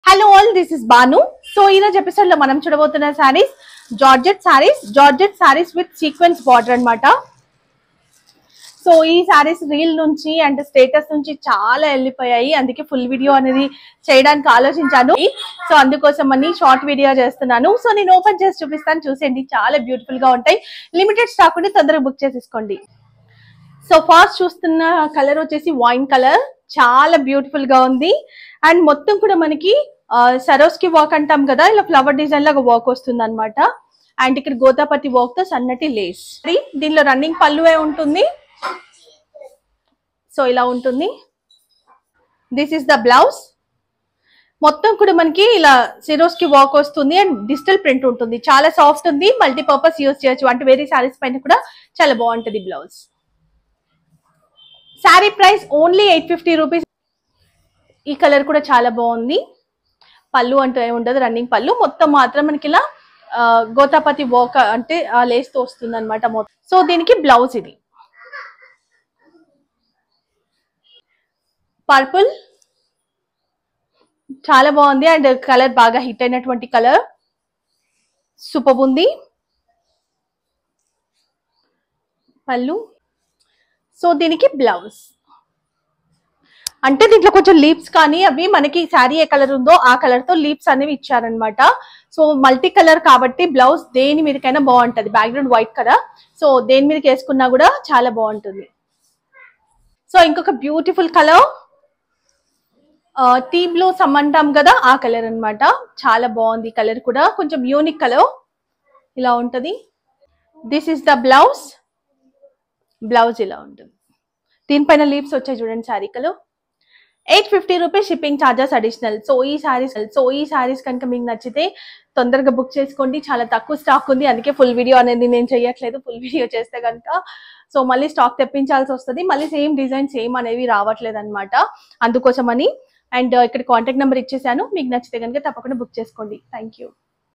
🌹Beautiful Georgette Designer Saree with Kalamkari Blouse 🌹Amma Selection ZB-08
This is Bhanu Priya from Hyderabad,
In Today Episode i will share Georgette Designer Sarees with Kalamkari Blouse Saree Collection

*No COD, Online Payment Phonepay and GPay
*Unboxing Video Compulsory 360° for return or exchange